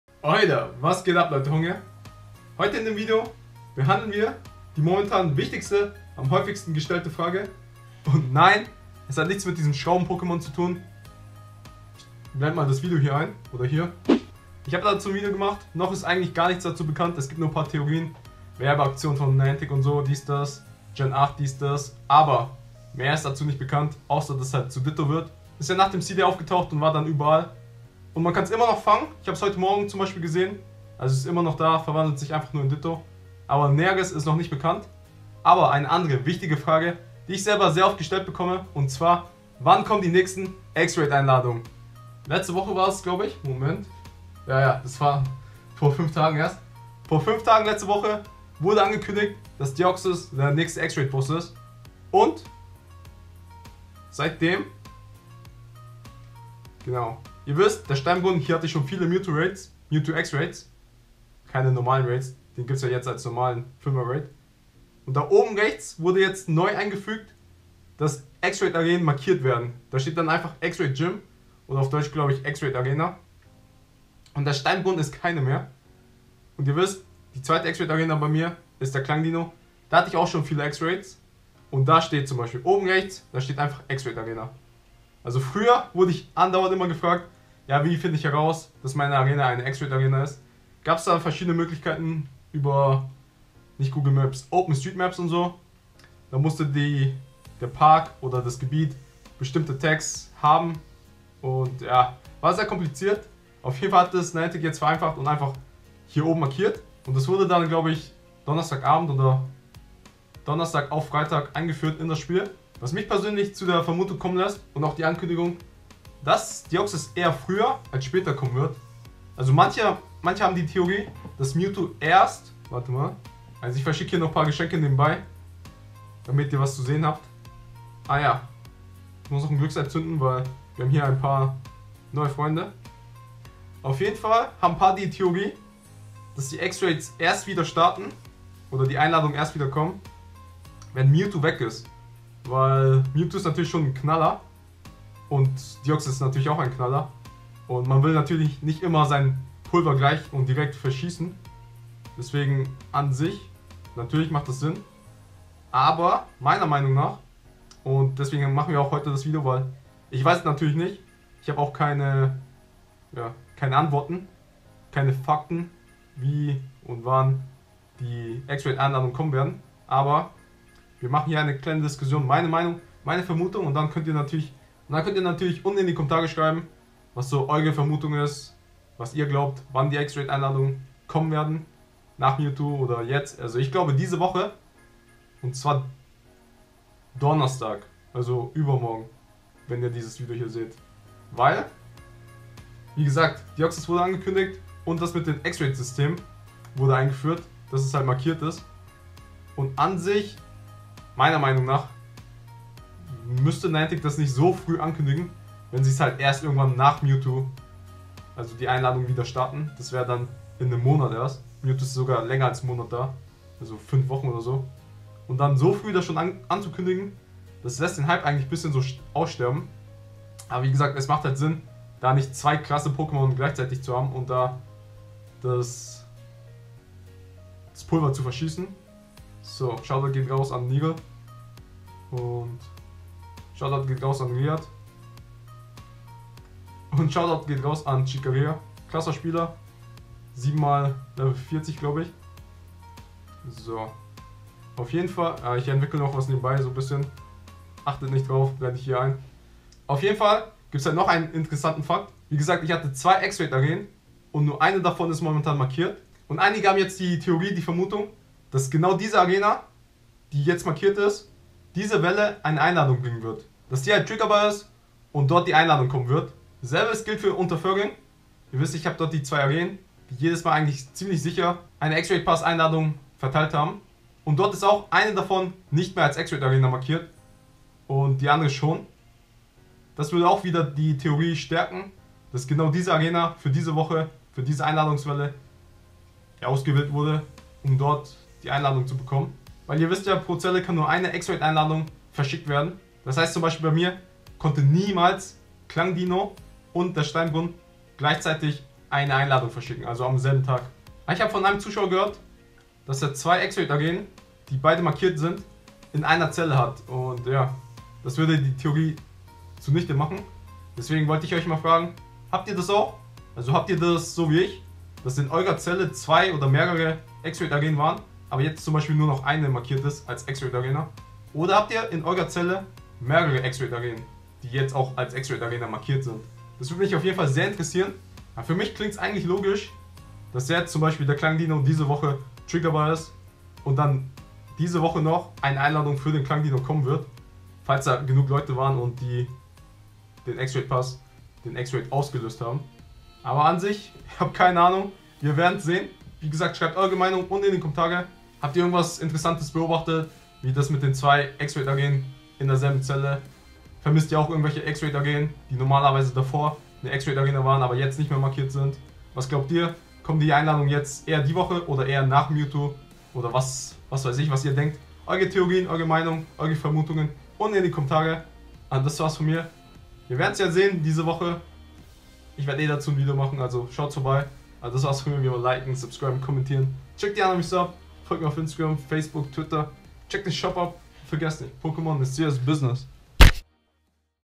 EX Raid Arenen sind VERSCHWUNDEN?! wann kommen die nächsten EX Raid Einladungen? Pokemon Go!
euda wann kommen die nächsten EX Raid Einladungen?
mit dieser Frage beschäfitge ich mich heute in dem Video
wann glaubt ihr kommen die neuen EX Raid Einladungen? sind bei euch auch EX Raid Arenen verschwunden?